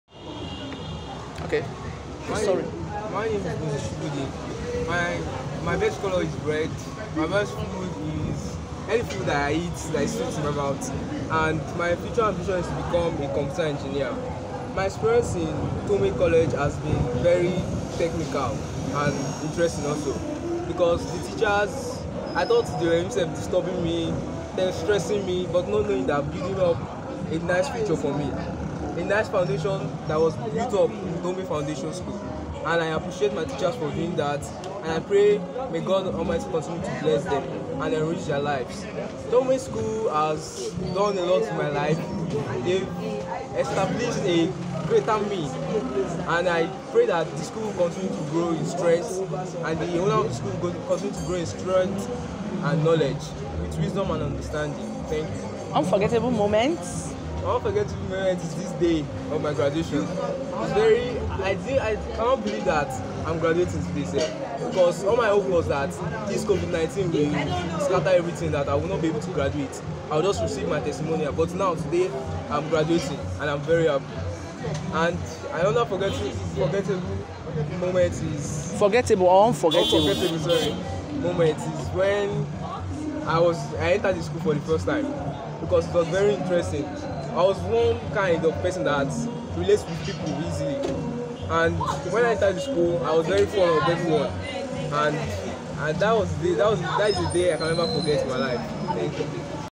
TOMBEY COLLEGE
S S 3 graduands shares his experience